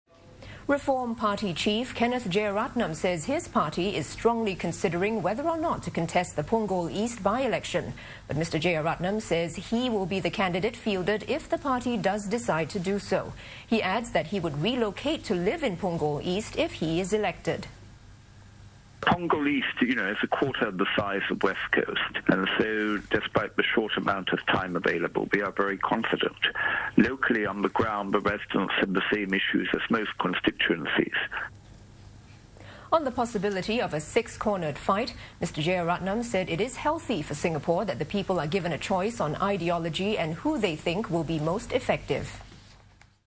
Reform Party mulling Punggol East by-election contest - 09Jan2013
SINGAPORE: Reform Party's Secretary-General Kenneth Jeyaretnam has said his party is "strongly considering" whether or not to contest the upcoming Punggol East by-election.

But Mr Jeyaretnam said he will be the candidate fielded if the party does decide to do so.

He added that he would re-locate to live in Punggol East if he is elected.

"Punggol East you know is a quarter the size of West Coast, and so despite the short amount of time available, we are very confident. Locally on the ground, the residents have the same issues as most constituencies," he said.

On the possibility of a six-cornered fight, Mr Jeyaretnam said it is healthy for Singapore that the people are given a choice on ideology and who they think will be most effective.

Chairman of the Singapore Democratic Party (SDP) Mohamed Jufrie Mahmood said news of the by-election being called for Punggol East came as a surprise.

He said the SDP did not expect that the Prime Minister would decide on the by-election so soon.

Speaking to the media at the Punggol East ward on Wednesday evening, Mr Jufrie said even though the party is ready for a contest, it would have to accelerate its campaign.

He said the party would be announcing its candidate within the next few days.

Mr Jufrie added that as far as possible, the party would try to avoid a multi-cornered fight.

He said that his party hopes to persuade the Workers' Party to give the voters the option of electing another opposition party.

But he added that if that does not happen, the SDP is firm on contesting the by-election.

Secretary-General of the Singapore Democratic Alliance (SDA) Desmond Lim said the news of the by-election date came as no surprise, and that the party is "operationally-ready".

He said it would be better for Punggol East SMC to have a parliamentary representative as soon as possible, rather than a caretaker one.

Mr Lim confirmed that the SDA would be fielding a candidate but declined to reveal a name for now.

Mr Lim said: "Well, it's good to have more than two parties in the Parliament. Parliament needs to have a diversified voice, and this will prevent, in case, the two parties come into coalition, there will still be other opposition voices in Parliament."